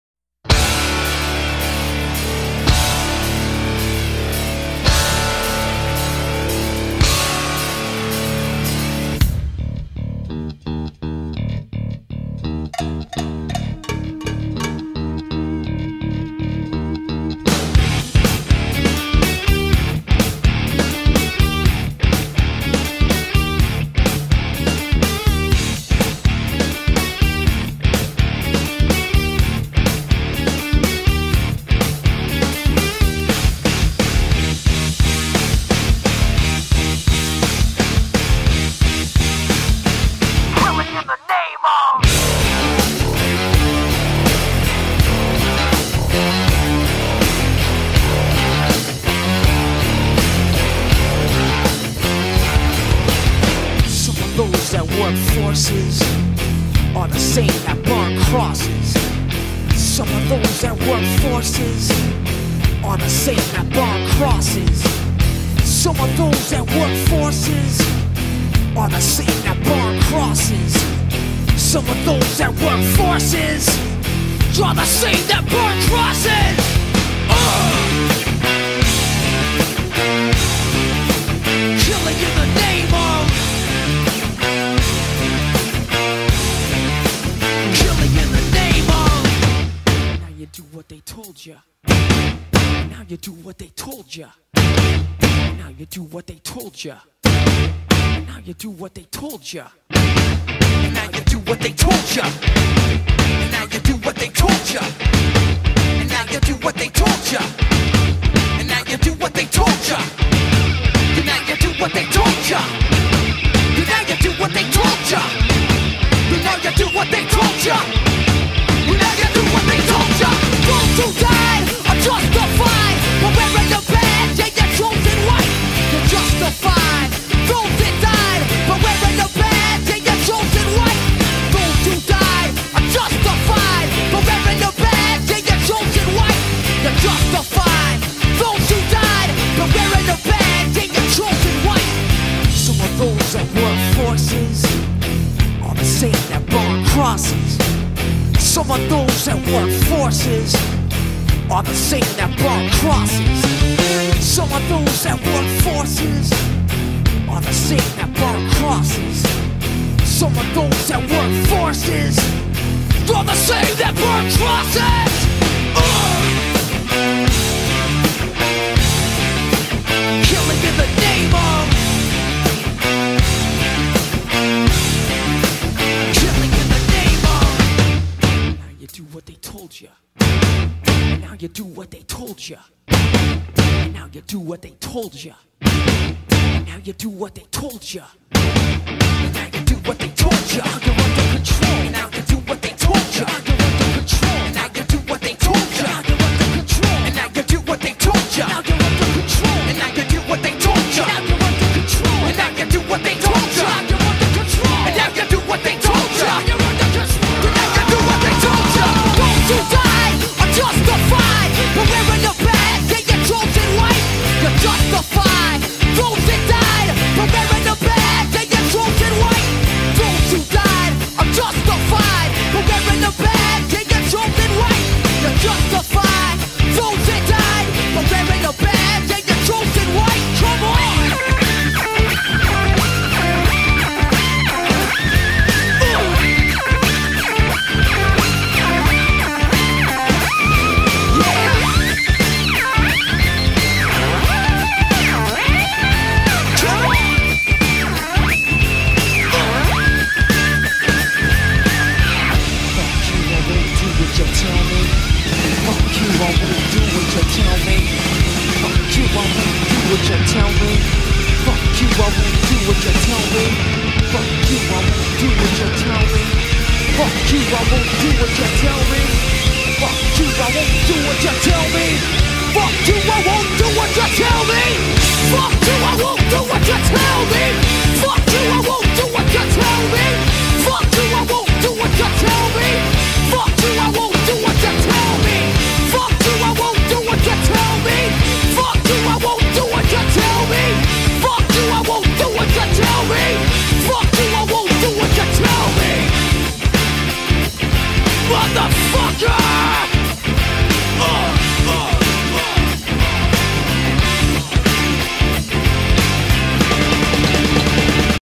STEPHANO photoshop painting by NICOLAS CHAUSSOIS
Making of Stephano art, for his departure of Millenium.

Art and video : Nicolas Chaussois
Technics : Handrawing, Watercolor, photoshop painting.